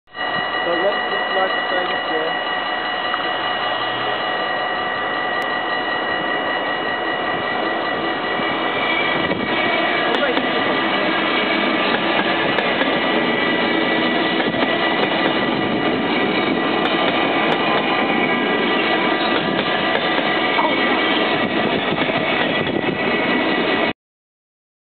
Treinpassage Dordrecht Zuid
Op vrijdag 27 maart 2009 kwam deze VIRM langzaam voorbij omdat  voor hem nog een andere trein reed.